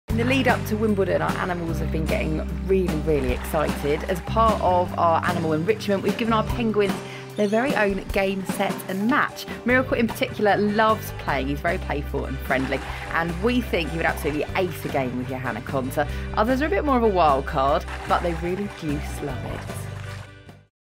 Drusillas Penguins take on Wimbledon
Penguins at Drusillas Park enjoy a Wimbledon themed enrichment!